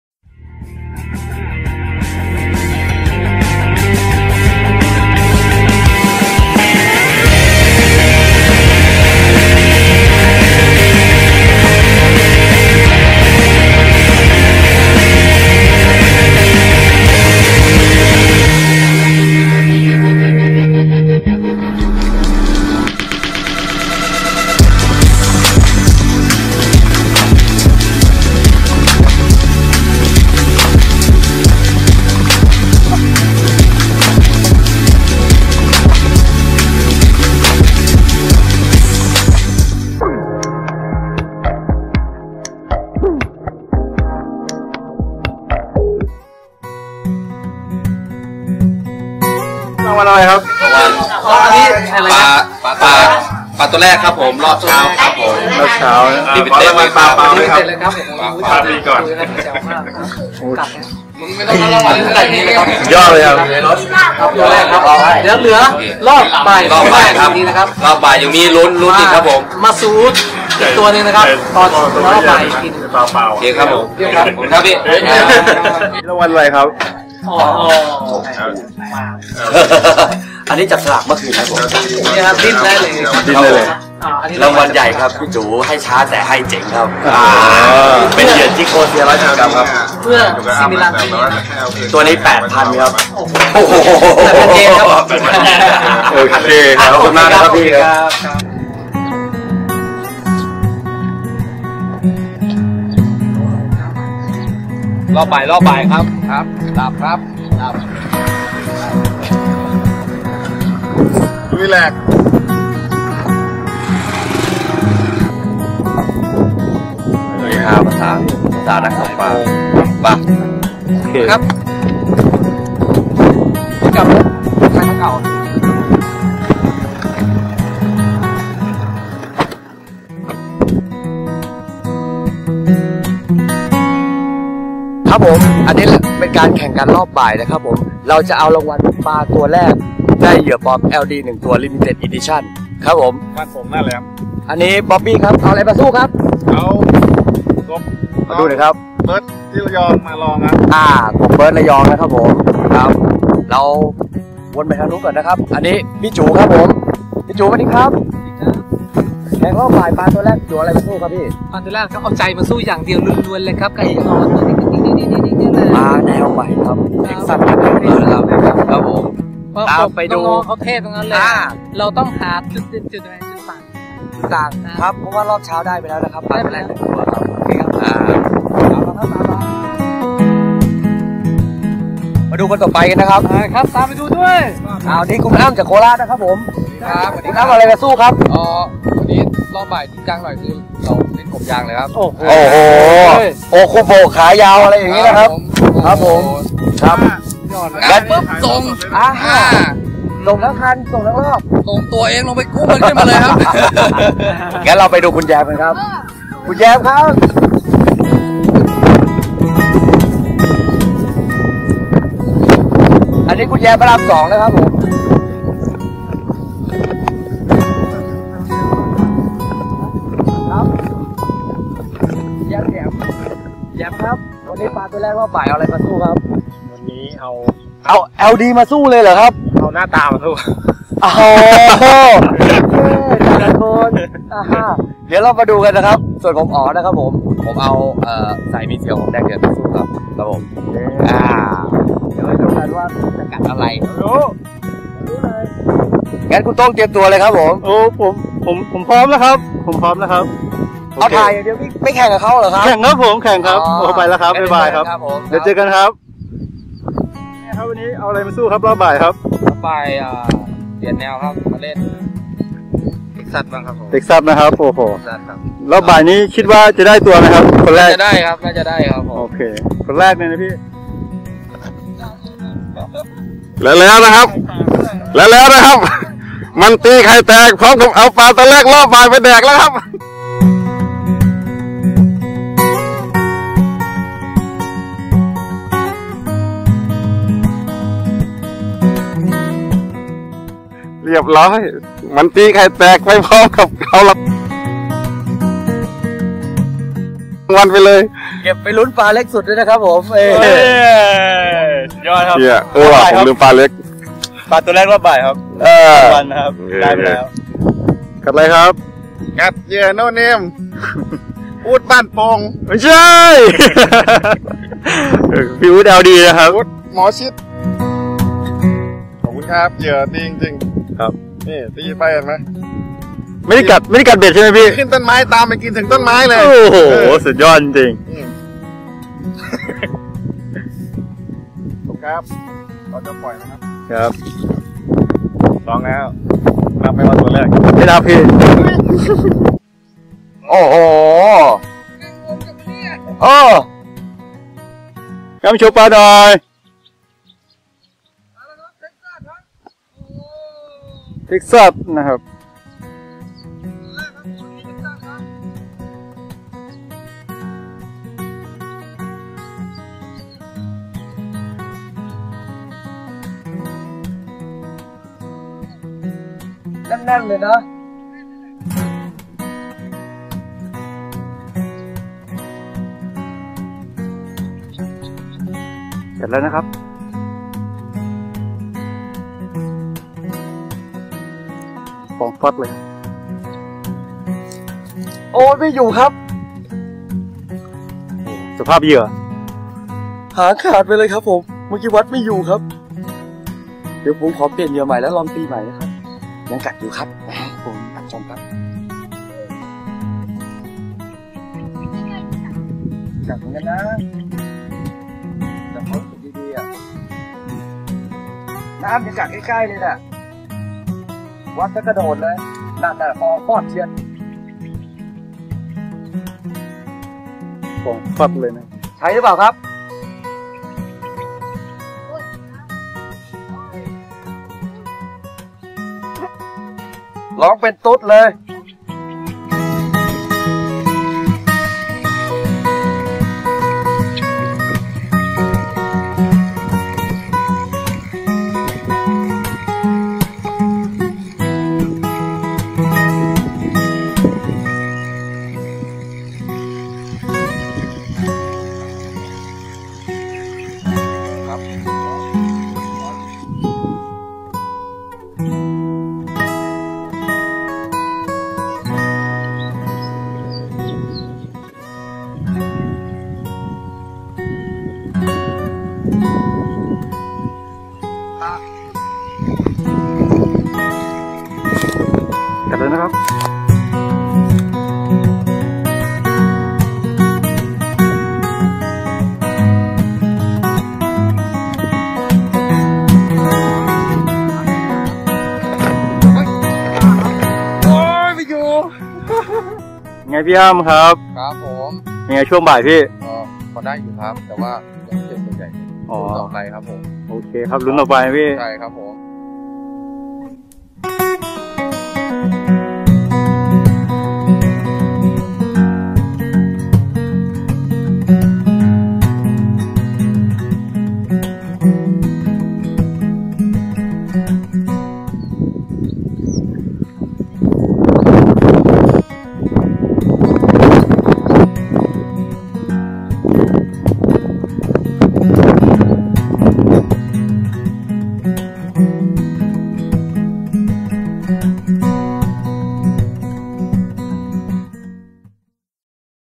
Fish Hunter EP : 6 ตกปลาช่อน บ่อสลิด 2558 #2
เมื่อวันที่ 1 กุมภาพันธ์ 2558 กลุ่มตกปลา Fishhunter ได้จัดทริปตกปลาช่อน บ่อสลิด 2558 แข่งขันภายในกลุ่ม สนุกสนานกัน เป็นทริปที่จัดขึ้นปีละ1ครั้ง ไม่แน่อาจจะมีทริปที่ 2 เนื่องจากสมาชิกภายในกลุ่มไม่ครบทีม มันสุดยอดมากทริปนี้ เชิญติดตามชมครับ